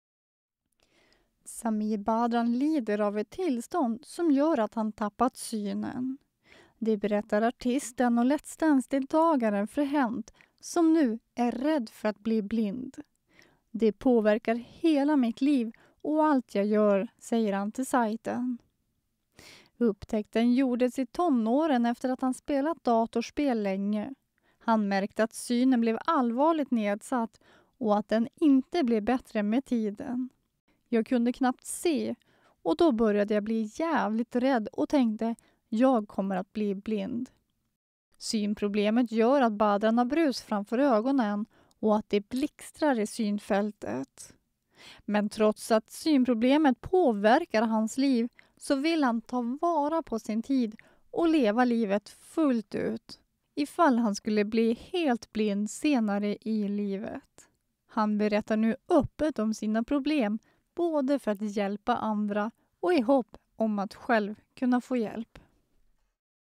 Samir Badran tror att han kommer bli blind
Samir Badran lider av ett tillstånd som gör att han tappat synen. Det berättar artisten och Let´s Dance deltagaren för Hänt.